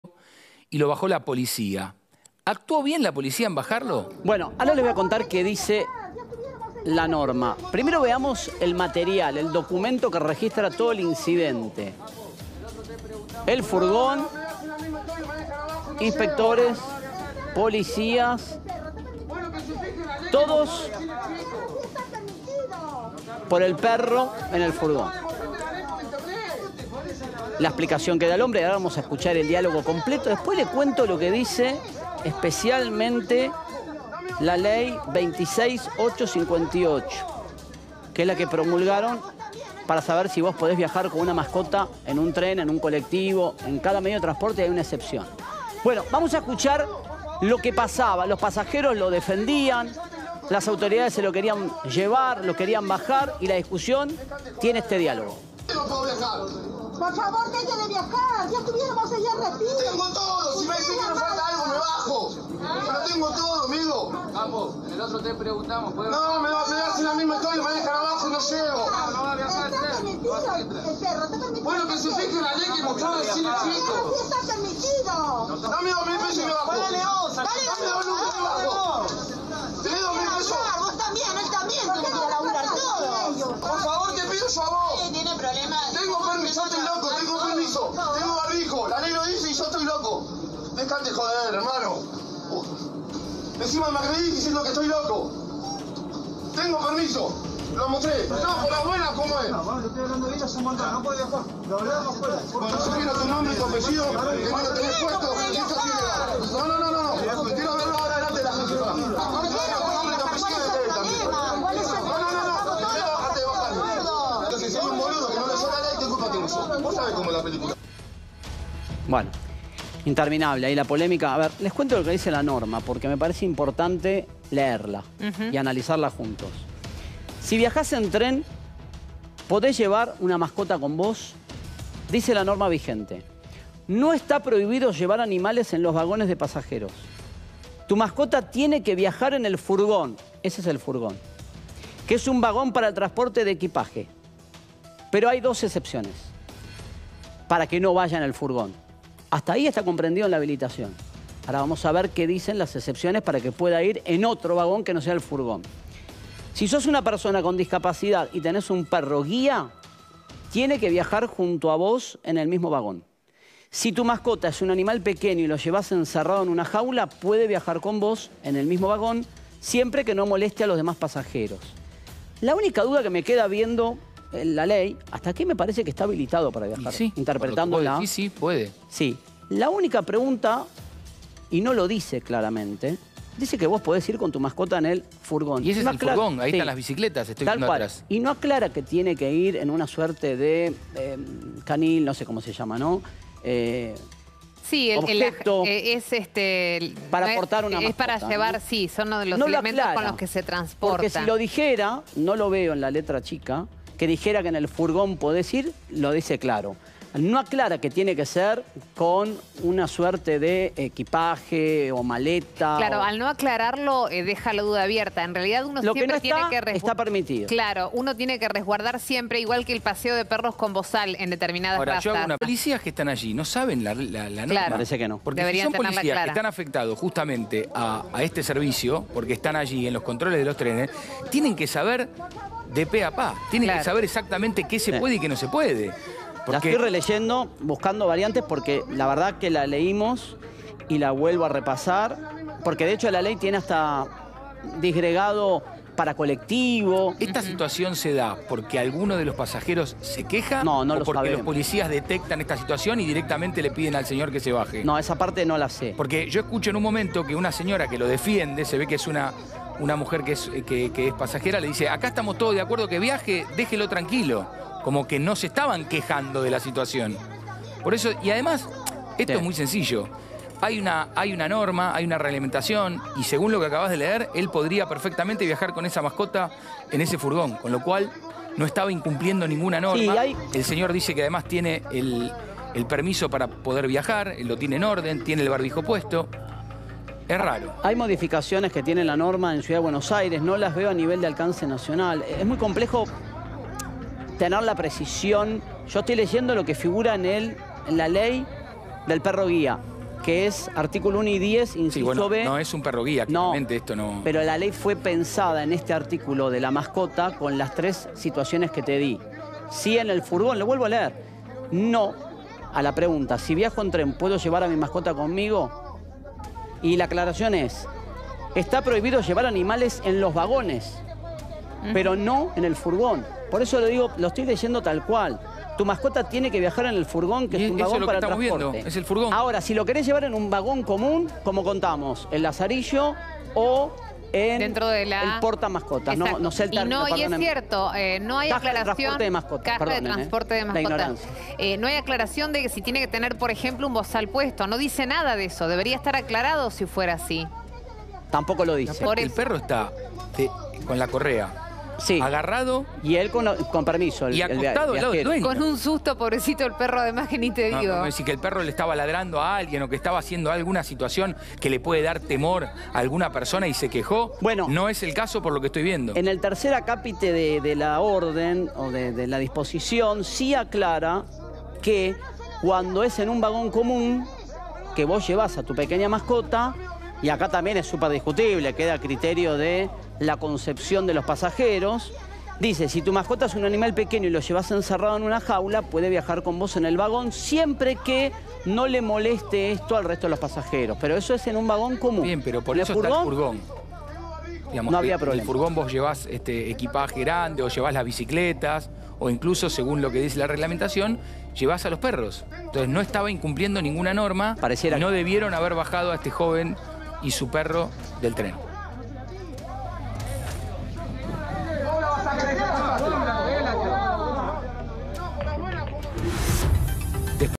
LO BAJARON DEL TREN POR VIAJAR CON SU PERRO
Una polémica situación se generó arriba del furgón de un tren.
Un pasajero viajaba con su perro y oficiales de la policía lo hicieron bajar a la fuerza.
El resto del pasaje defendía al hombre ya que el animal no estaba causando ningún problema y tenía correa. Lo llamativo es que mientras la ley vigente permite sin problemas viajar con una mascota en el furgón, los oficiales de la policía se lo hayan impedido.